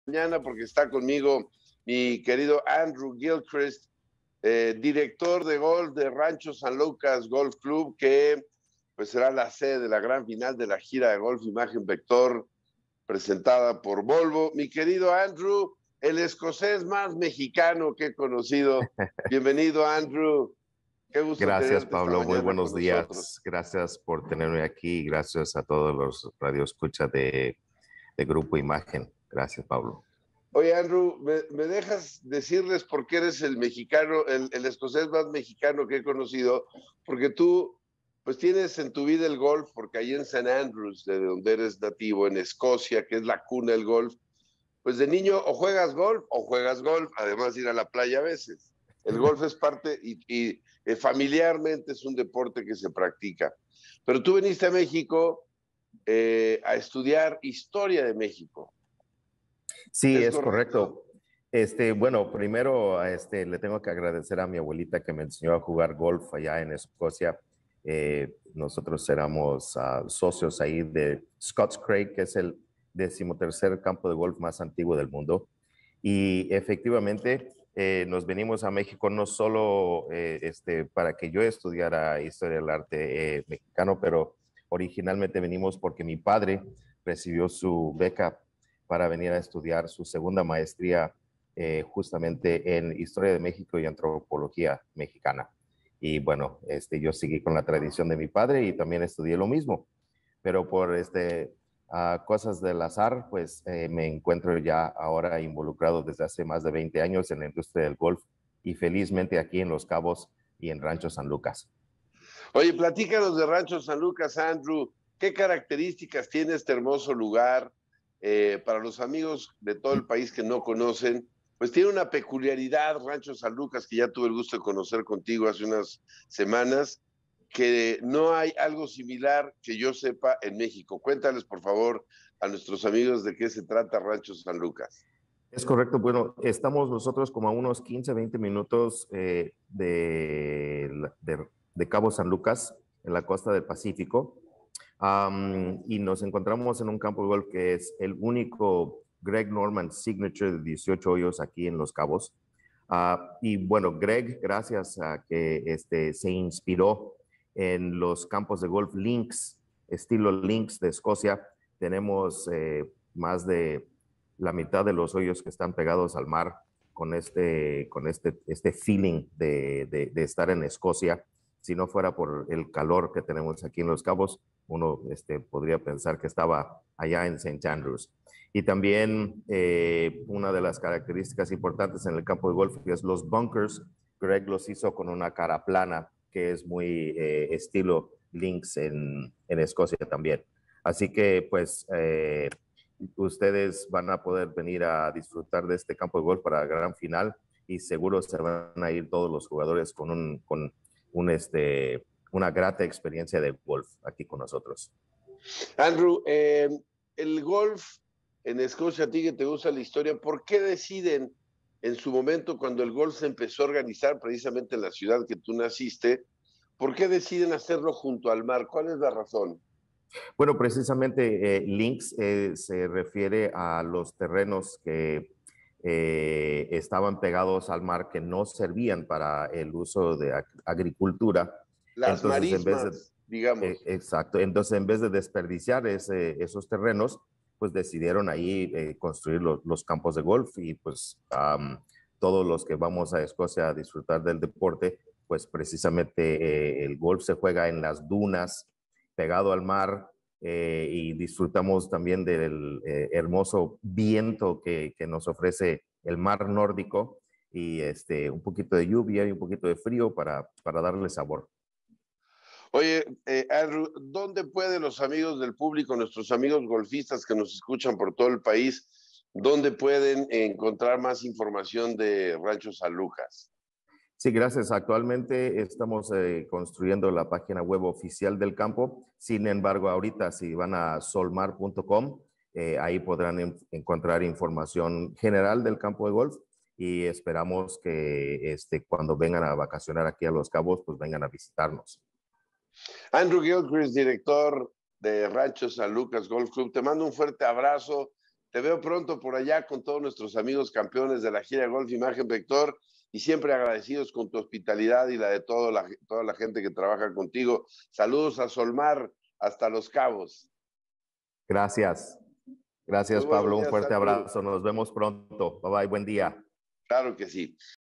Andrew Gilchrist, el escocés más mexicano | Palabra del Deporte
AndrewGilchrist, director de golf, nos acompañó en Imagen Radio. Gilchrist nos contó todo sobre dicho deporte, aquí la entrevista.